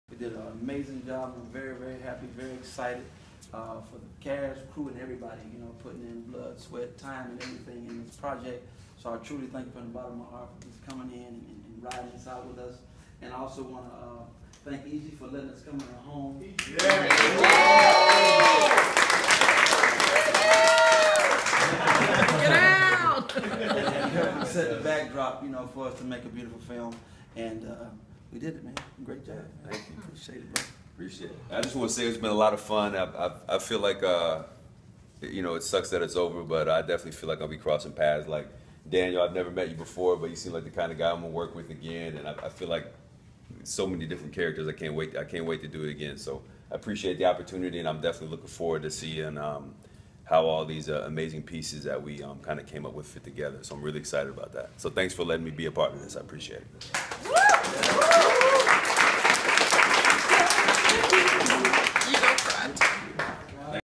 It's a Wrap!
Director Alton Glass and actor McKinley Freeman on wrap day of the upcoming TVOne Christmas movie, "Second Chance Christmas."